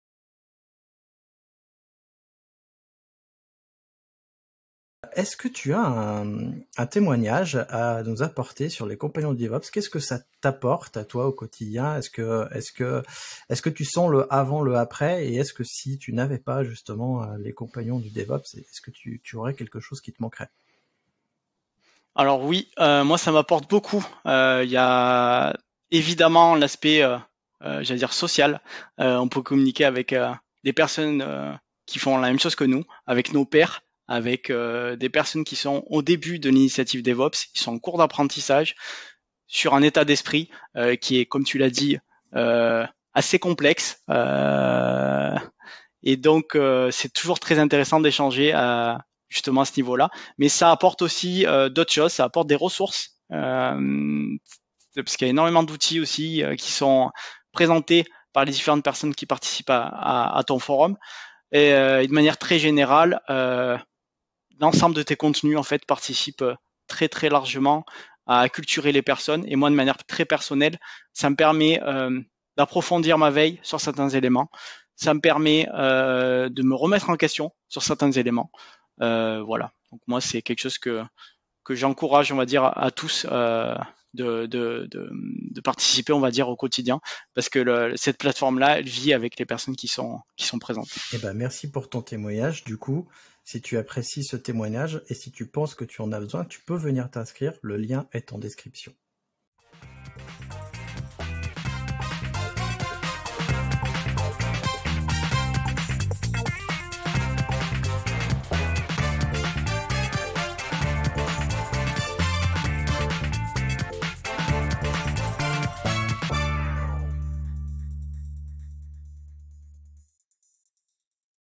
Témoignage de Daniel Castronovo sur les Compagnons du DevOps
Daniel Castronovo est SRE chez Yousigne et témoigne de ce que lui apporte les Compagnons du DevOps.